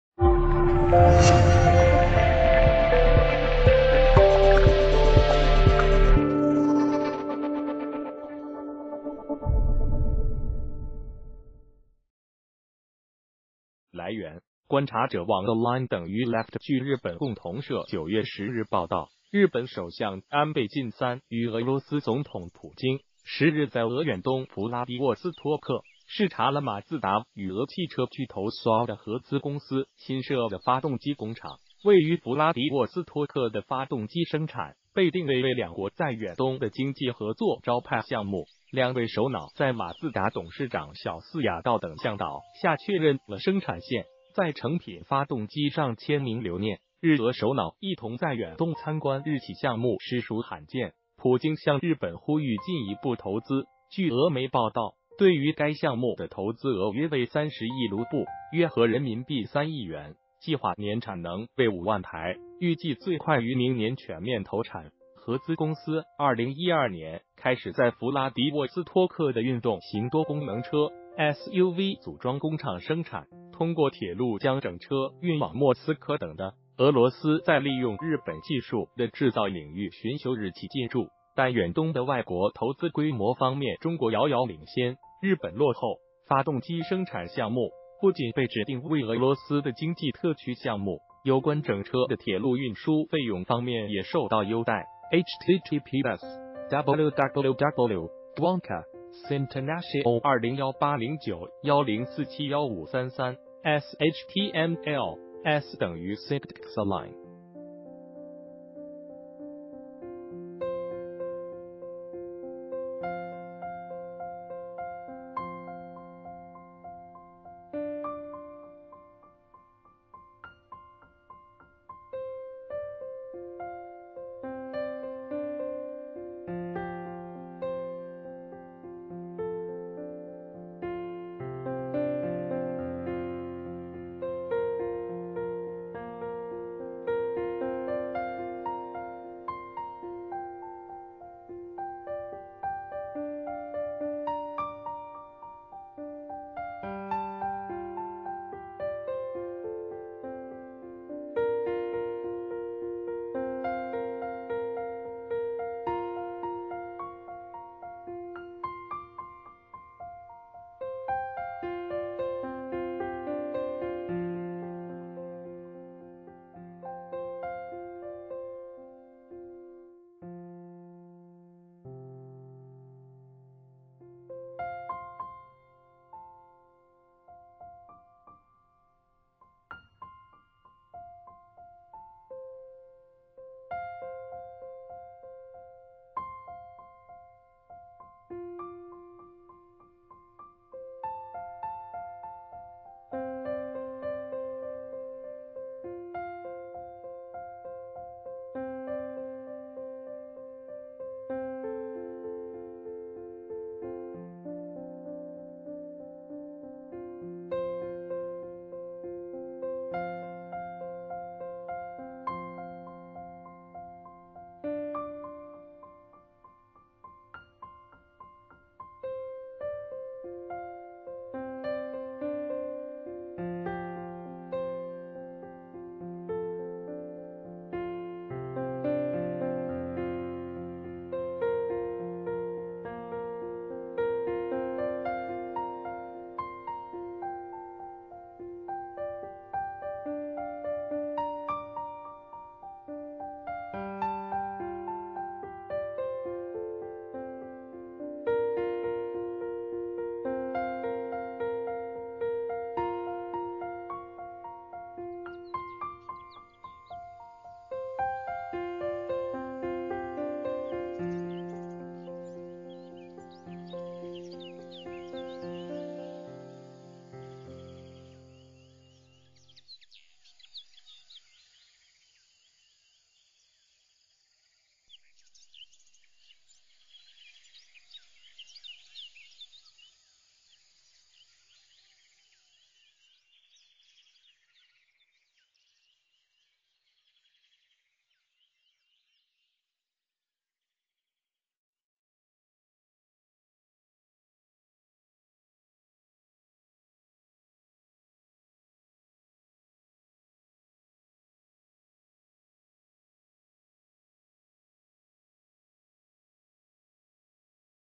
日俄,首脑,远东,视察,马自达,合资,公司,工厂
谢谢！来源：观察者网[align=left]据日本共同社9月10日报道，日本首相安倍晋三与俄罗斯总统普京10日在俄远东符拉迪沃斯托克，视察了马自达与俄汽车巨头Solle的合资公司新设的发动机工厂。位于符拉迪沃斯托克的发动机生产被定位为两国在远东的经济合作招牌项目。两位首脑在马自达董事长小饲雅道等向导下确认了生产线，在成品发动机上签名留念。日俄首脑一同在远东参观日企项目实属罕见，普京向日本呼吁进一步投资。据俄媒报道，对于该项目的投资额约为30亿卢布（约合人民币3亿元），计划年产能为5万台。预计最快于明年全面投产。合资公司2012年开始在符拉迪沃斯托克的运动型多功能车（SUV）组装工厂生产。通过铁路将整车运往莫斯科等地。俄罗斯在利用日本技术的制造领域寻求日企进驻，但远东的外国投资规模方面中国遥遥领先，日本落后。发动机生产项目不仅被指定为俄罗斯的经济特区项目，有关整车的铁路运输费用方面也受到优待。https